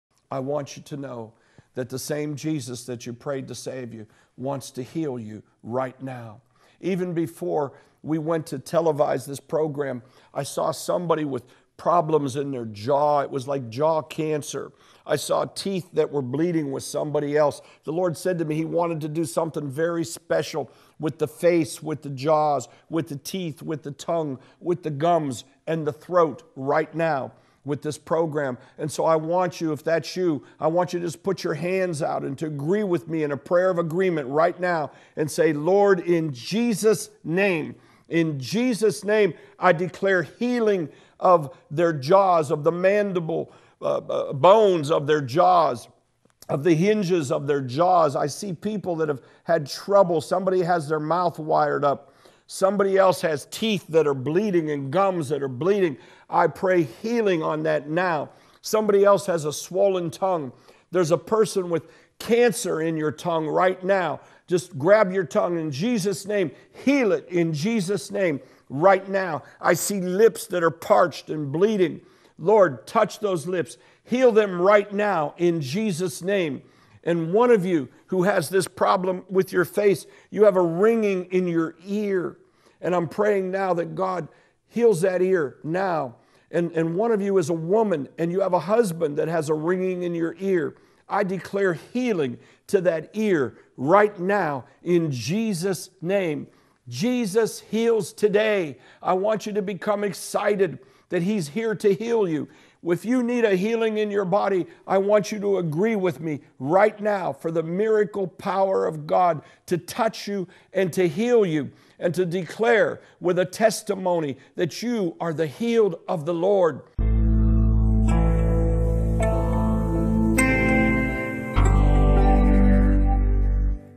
Trump's 'Christian Policy Liaison' Frank Amedia Conducts TV Faith Healings
Frank Amedia claiming to heal people over his TV broadcast